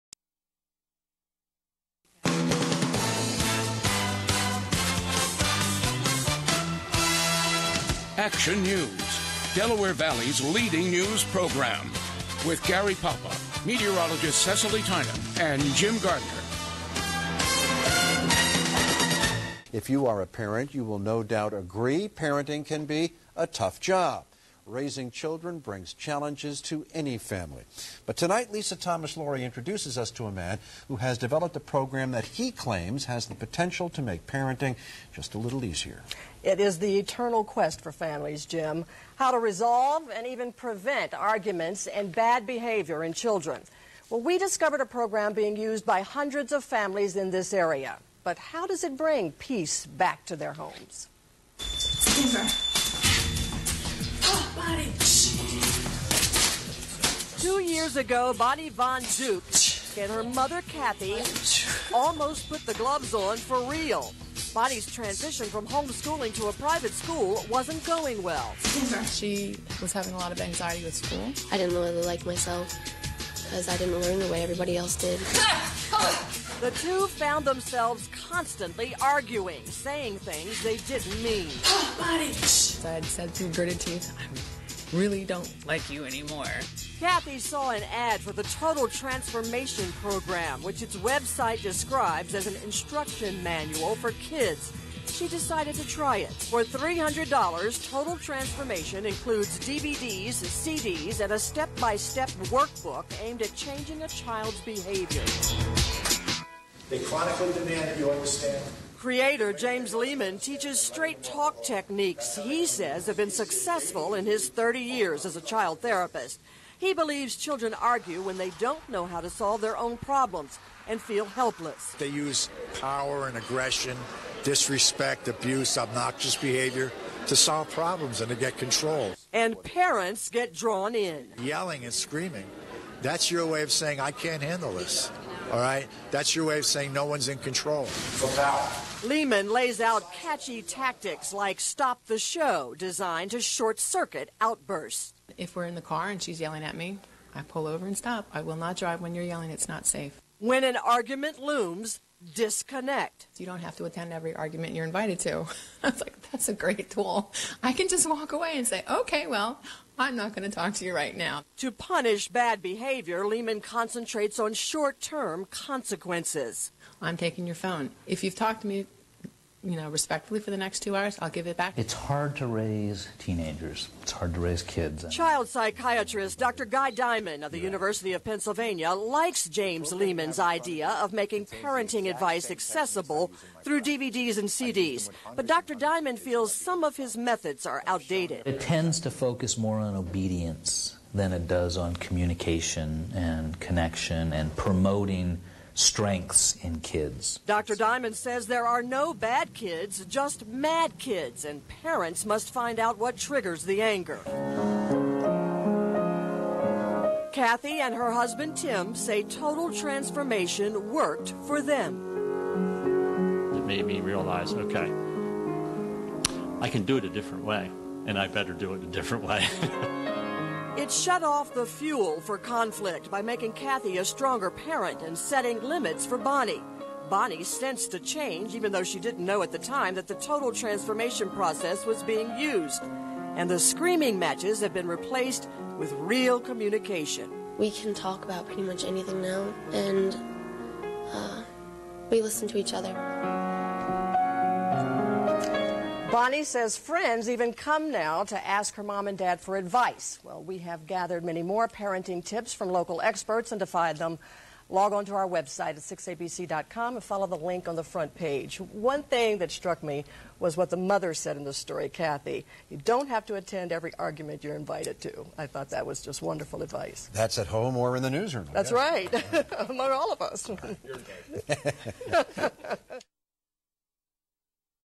The Total Transformation on WPVI
Child behavior program.  This was the new segment that led me to the program.  I'm glad I saw it.